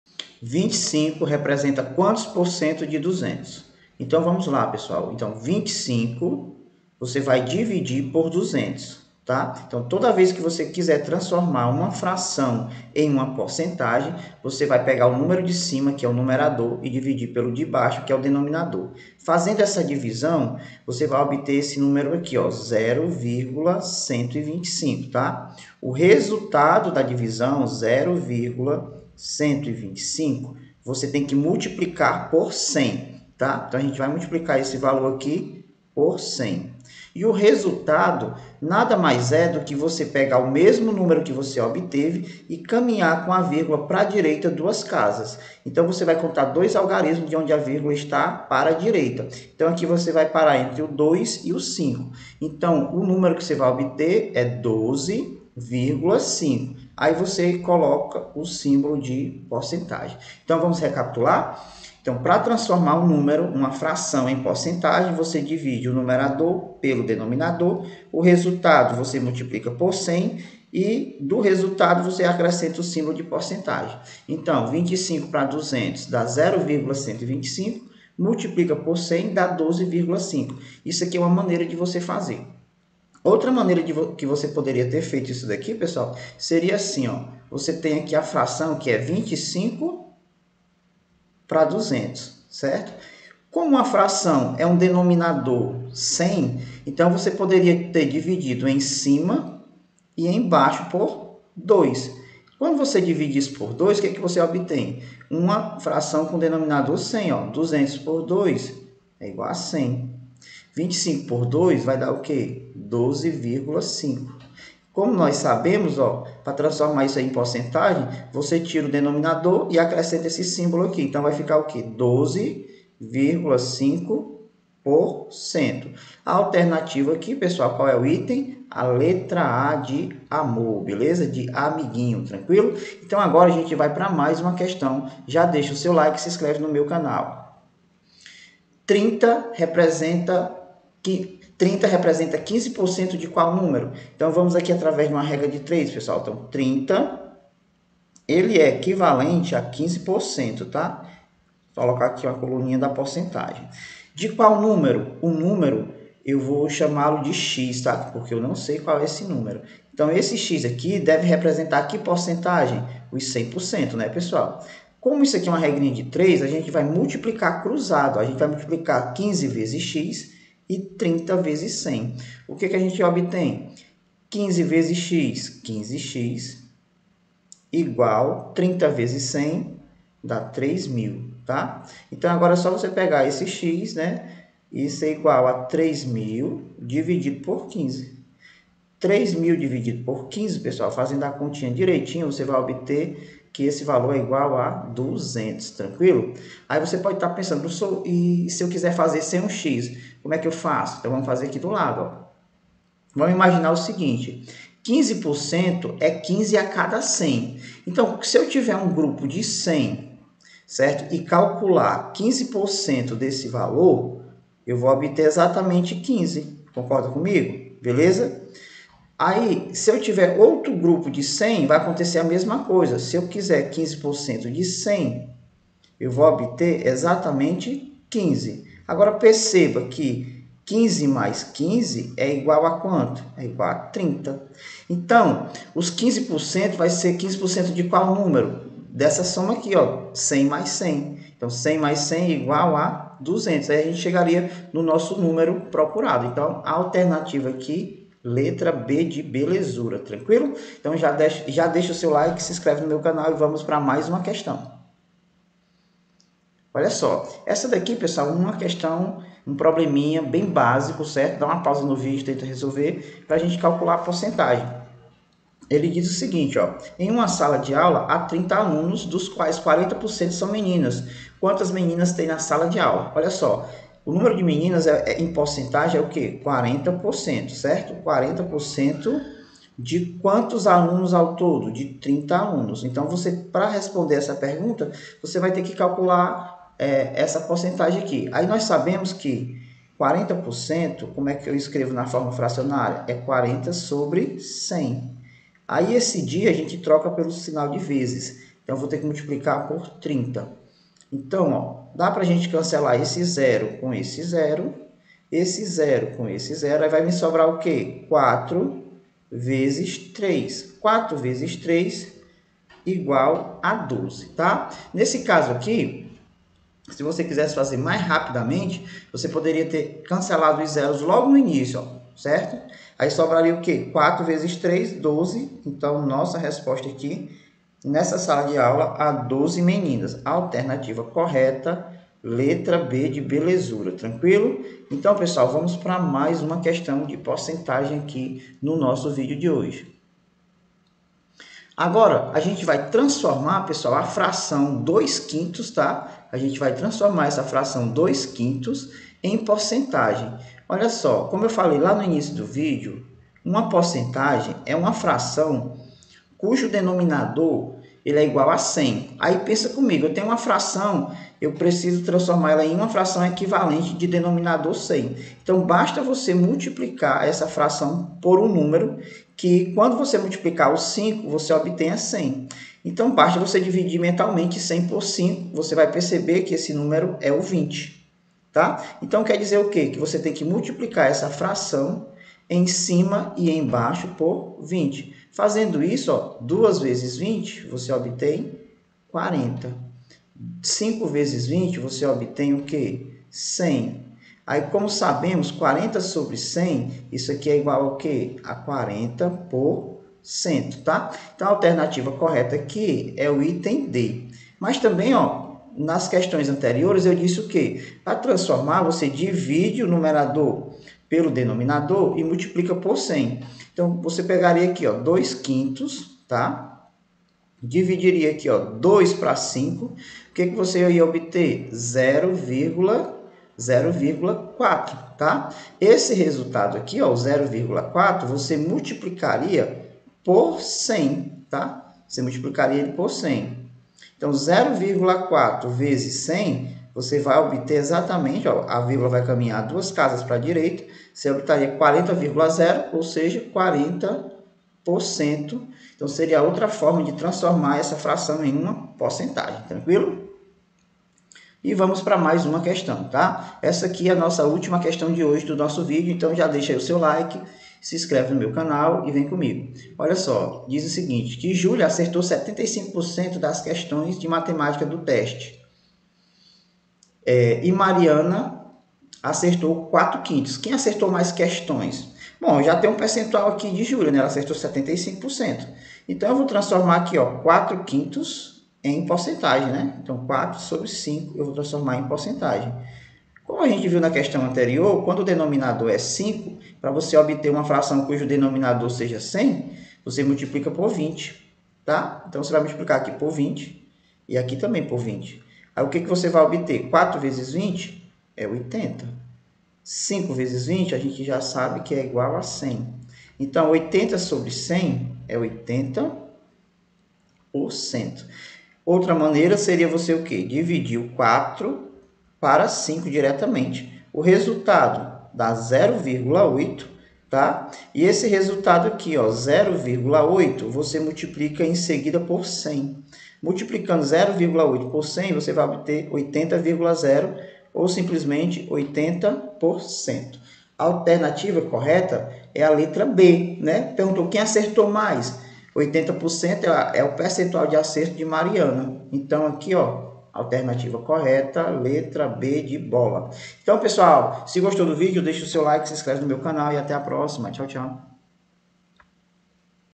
🏅Nunca Mais Erre Porcentagem! | Aprenda em 9 minutos 🤜🤛
Olá Pessoal!
A aula de hoje é sobre porcentagem, devemos lembrar que PORCENTAGEM é uma fração de denominador igual 100, ou seja, X/100. Para calcular uma porcentagem podemos escrever na forma fracionária ou decimal e multiplicar pelo número dado. Vamos resolver 5 exercícios envolvendo porcentagem.

1) 25 representa quantos por cento de 200?

2) 30 representa 15% de qual número?

3) Em uma sala de aula há 30 alunos, dos quais 40% são meninas. Quantas meninas têm na sala?

4) Convertendo a fração ⅖ em uma fração centesimal, qual o resultado em porcentagem?

5) Júlia acertou 75% das questões de Matemática do teste e Mariana acertou 4/5. Quem acertou mais questões?